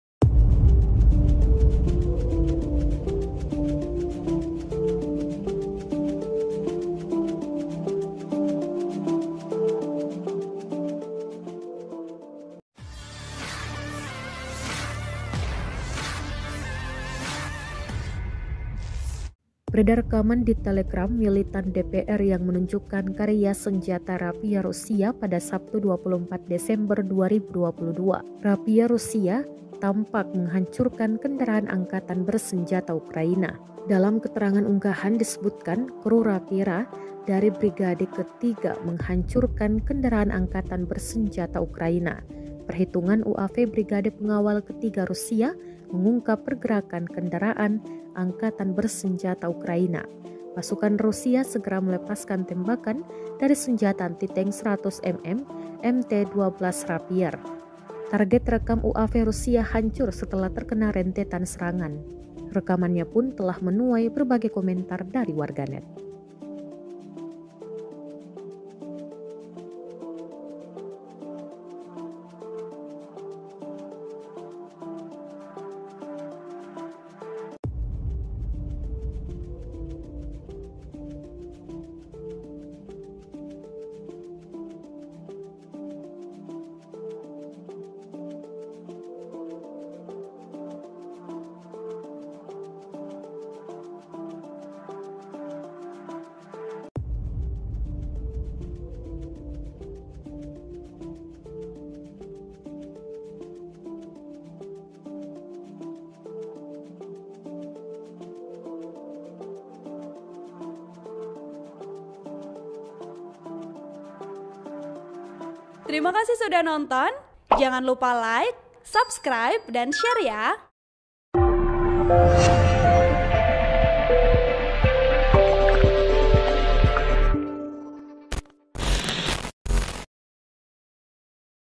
Senjata Anti-tank 100 mm MT-12 "Rapier" Rusia Hancurkan Kendaraan Lapis Baja Ukraina
TRIBUN-TIMUR.COM - Beredar rekaman di Telegram Militan DPR yang menunjukkan karya senjata "Rapier" Rusia pada Sabtu, 24 Desember 2022.

Rapier Rusia tampak menghancurkan kendaraan Angkatan Bersenjata Ukraina.

Dalam keterangan unggahan disebutkan Kru Rapira dari brigade ke-3 menghancurkan kendaraan Angkatan Bersenjata Ukraina.

Perhitungan UAV Brigade Pengawal ke-3 Rusia mengungkap pergerakan kendaraan Angkatan Bersenjata Ukraina.

Pasukan Rusia segera melepaskan tembakan dari senjata anti-tank 100 mm MT-12 "Rapier".

Target terekam UAV Rusia hancur setelah terkena rentetan serangan.

Rekamannyapun menuai berbagai komentar dari warganet.

Narator: Ririn